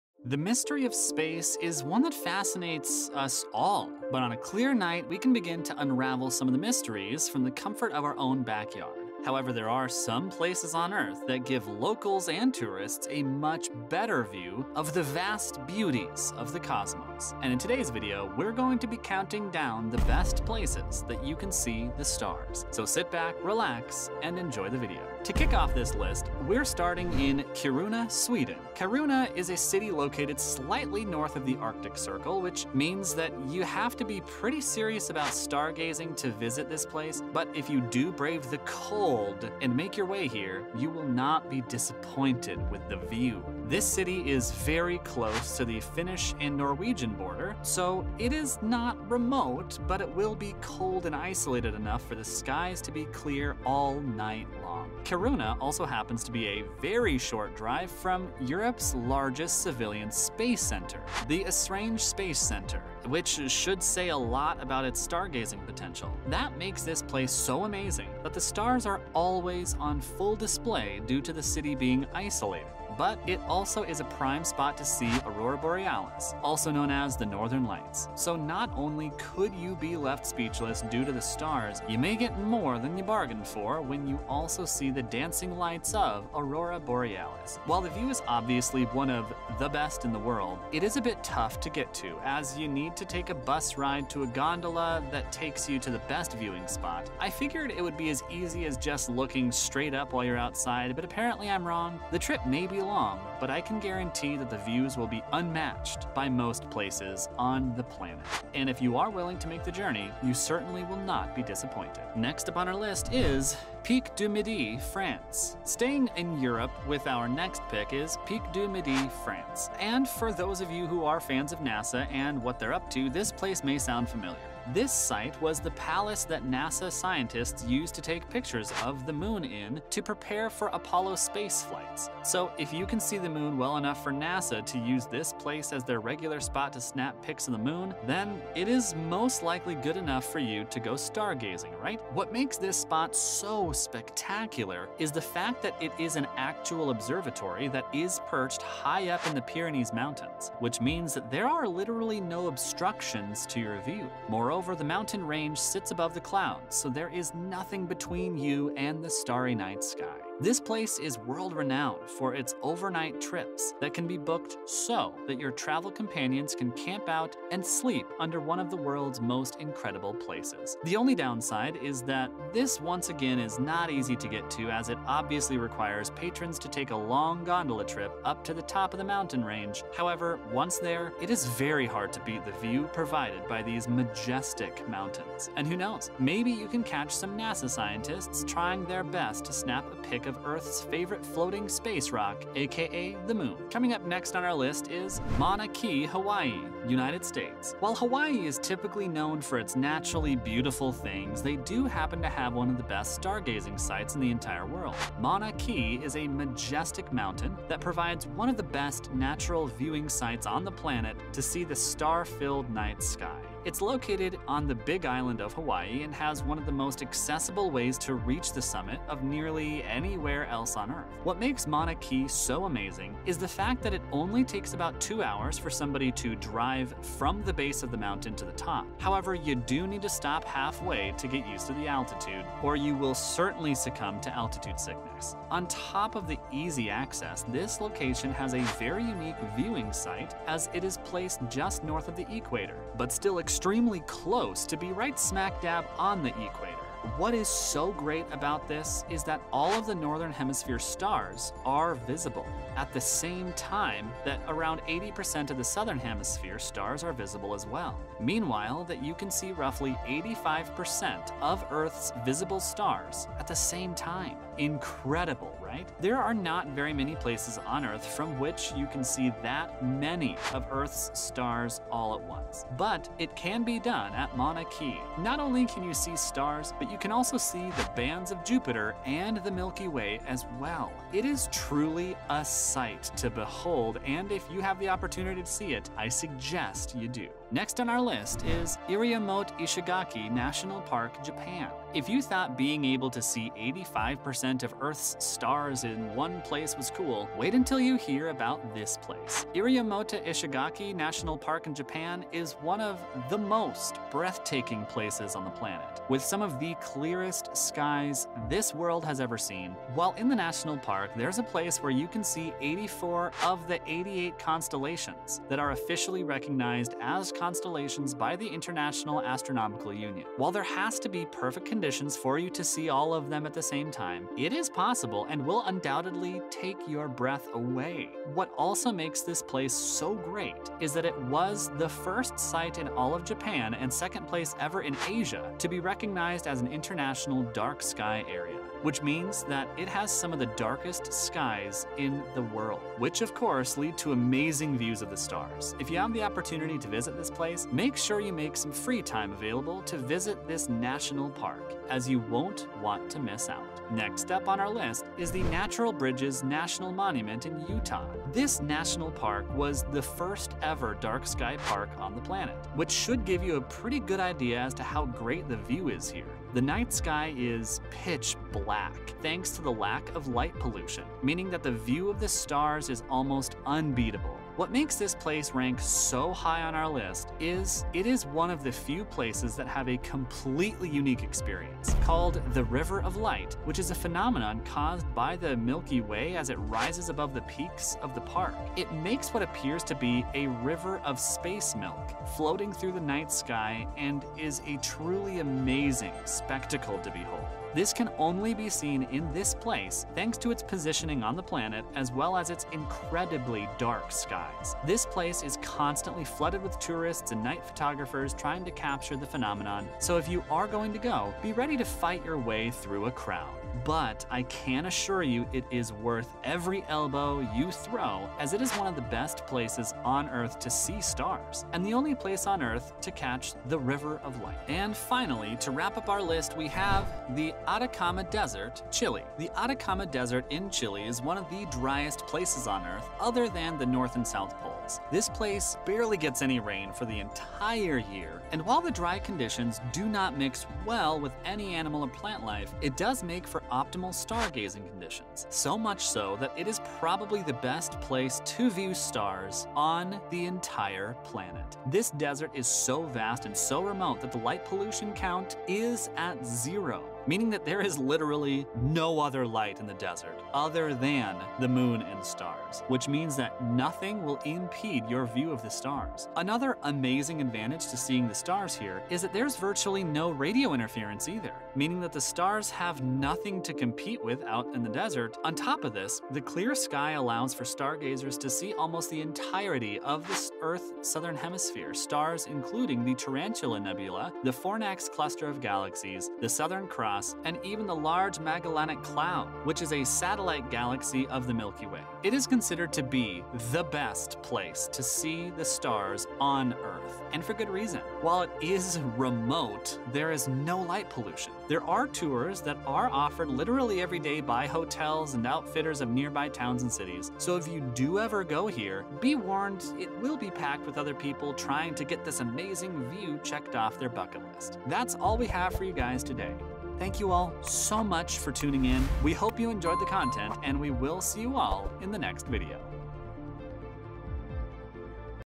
BEST Spots Around The World To Stargaze..
Welcome back to Outdoor Central. The immensity of the universe is beautiful and humbling—the stars are reminders of billions of lives spent, in astronomer Carl Sagan’s words, “on a mote of dust, suspended in a sunbeam.” But as our population grows, so too does light pollution, clouding the atmosphere and all that lies beyond it. Today, as few as 500 stars are visible from many urban areas.

But happily that's just not the case for these locales, where as many as 15,000 individual pinpricks of light can be seen with the naked eye. The catch? They're not exactly convenient—but that's kind of the point.

15 interesting objects in the night sky at the same time!! Autumn 2021 Planets, Comets, Galaxies...